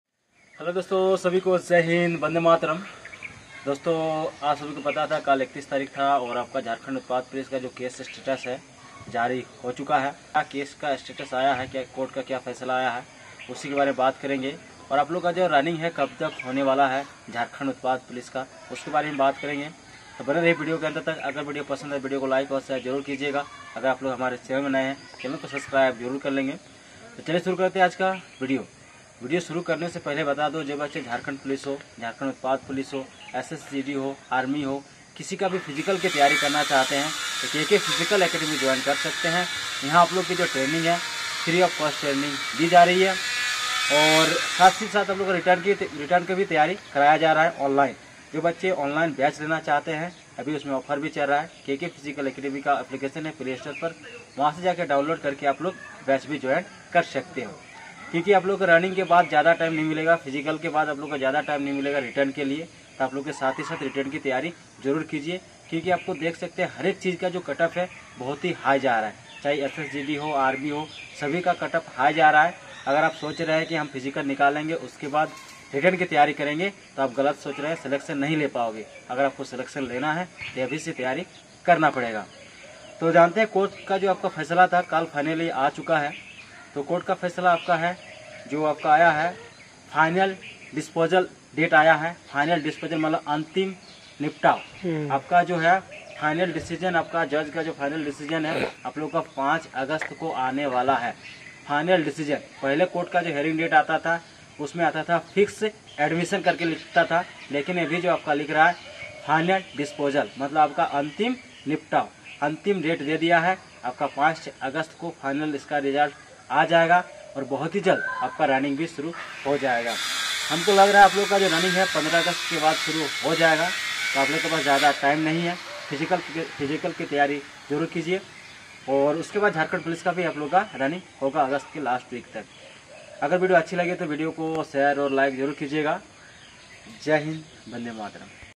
Utpad Sipahi Case update || कब होगी दौड़ || Final Disposal Date
For those following the Utpad Sipahi recruitment process, recent updates have provided some clarity. The latest **Utpad Sipahi case update** reveals that the authorities are diligently processing applications and addressing various concerns. Many are wondering, "**Utpad Sipahi ka Admit card kab ayega**," and while a specific date hasn’t been confirmed yet, candidates are advised to regularly check the official website for announcements. As for the **Utpad Sipahi Running update**, it’s essential for candidates to stay prepared and keep an eye on any changes to the physical test schedule. Staying informed through official channels will ensure you’re up-to-date with all developments.